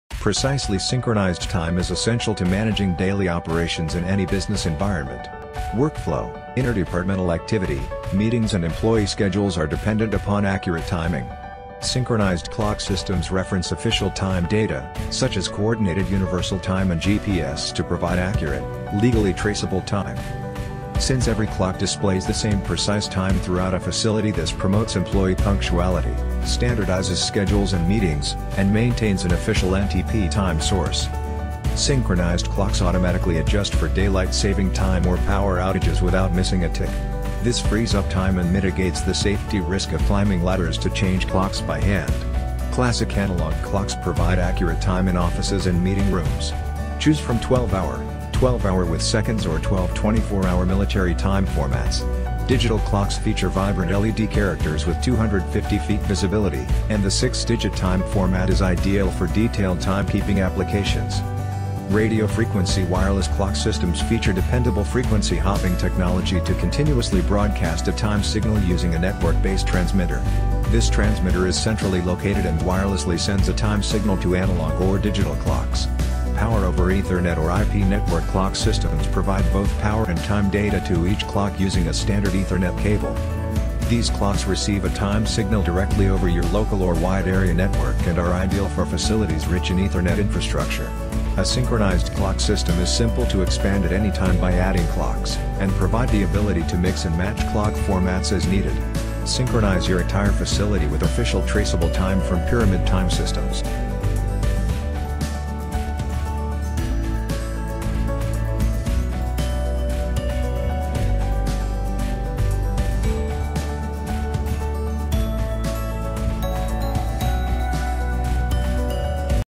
Synchronized Wall Clocks for Business
Workflow, interdepartmental activity, meetings, deadlines and employee schedules are dependent upon accurate timing. Synchronized clock systems reference official time data, such as Coordinated Universal Time and GPS to provide accurate, legally traceable time.
Since every clock displays the same precise time throughout a distribution facility, this promotes employee productivity, standardizes schedules and meetings, and maintains an official NTP time source.
Synchronized clocks automatically adjust for daylight saving time or power outages without missing a tick. This frees up time and mitigates the safety risk of climbing ladders to change clocks by hand. Classic analog clocks provide accurate time in classrooms and administrative areas.  Choose from 12 hour, 12 hour with seconds or 12 24 hour time formats. Digital clocks feature vibrant LED characters with 250ft visibility, and the 6 digit time format is ideal for detailed timekeeping applications.
Count up and countdown digital timers precisely monitor elapsed or remaining time, providing time measures for emergency operation plans. Radio Frequency Wireless Clock Systems feature dependable frequency-hopping technology to continuously broadcast a time signal using a network based transmitter. This transmitter is centrally located and wirelessly sends a time signal to analog or digital clocks. Power over Ethernet or IP Network Clock Systems provide both power and time data to each clock using a standard Ethernet cable.
These clocks receive a time signal directly over your local or wide area network and are ideal for facilities rich in Ethernet infrastructure. A Synchronized clock system is simple to expand at any time by adding clocks, and provide the ability to mix and match clock formats as needed. Synchronize your entire facility with official traceable time from Pyramid Time Systems.